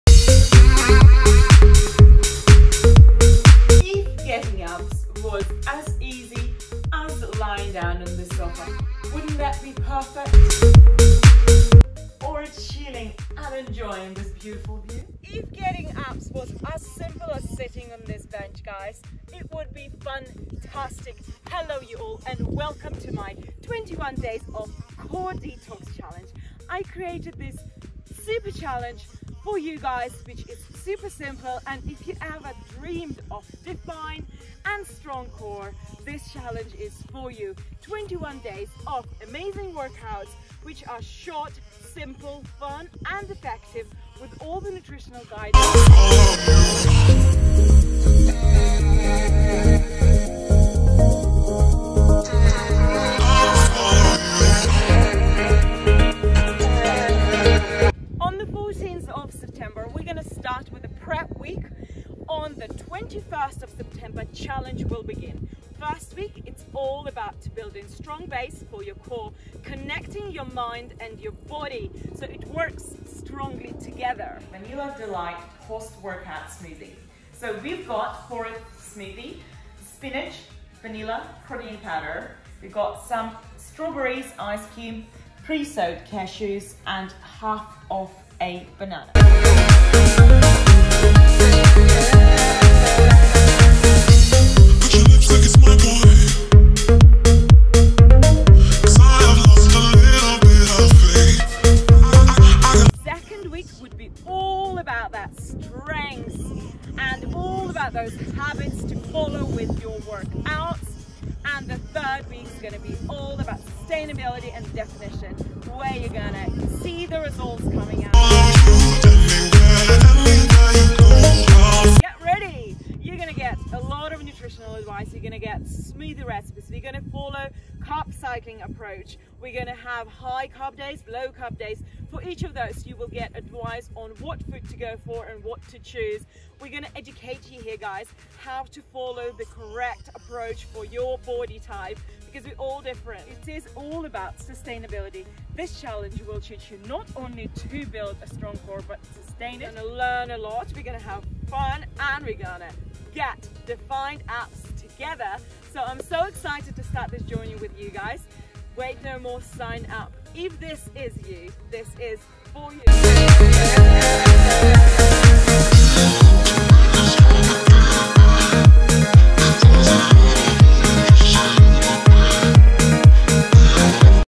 Core Detox 21 Challenge
Core Detox 21

Starting on the 21st of September

Preparation week to get you ready for the start of the challenge on the 14th of September

When you sign up you will be added to a private Facebook Page where the magic will happen 💫

Week #1 Building the foundations
Week #2 Working on developing your core
Week #3 Shredding fat

Focusing on teaching you how to get the results you want, guiding you through the process one step at a time giving you all the necessary tools to sustain your results after completing the CoreDox21 challenge.

Six days a week, five 20 min live workouts, one 45min live session with 24 hours to complete them! In addition to a prerecorded daily 5min core focus workout for you to fit into your schedule! Treat it like a snack 😉 there for when you want it !!